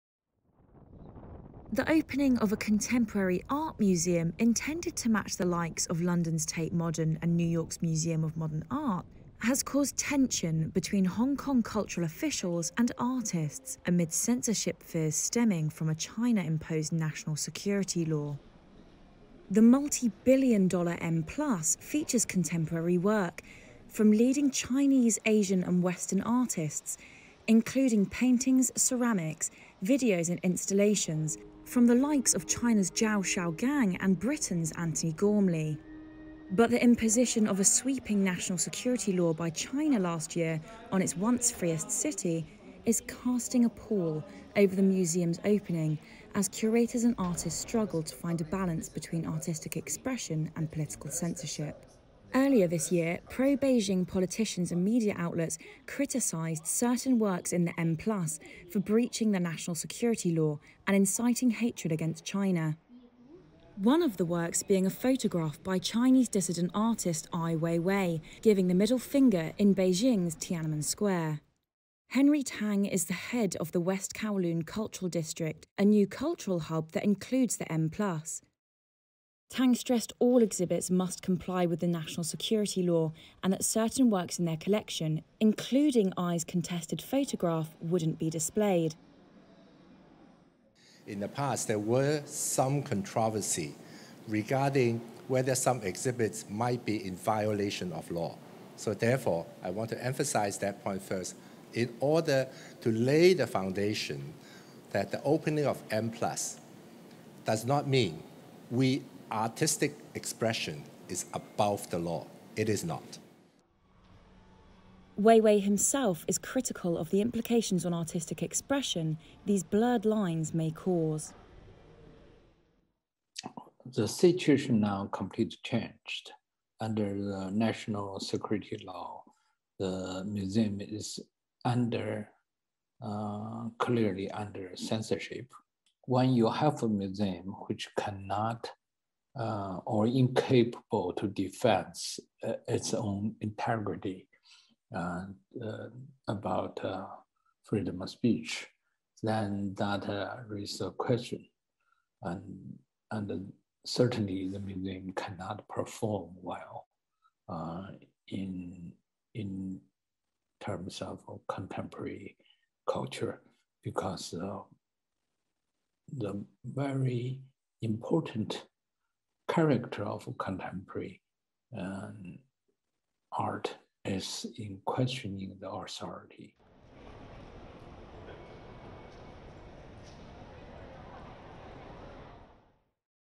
Hong Kong art museum faces censorship backlash
A senior Hong Kong cultural official said on Thursday (November 11) that freedom of expression was not above a China-imposed national security law, during a preview ahead of the opening of a contemporary art museum intended to put the city on the global cultural map.

VIDEO: REUTERS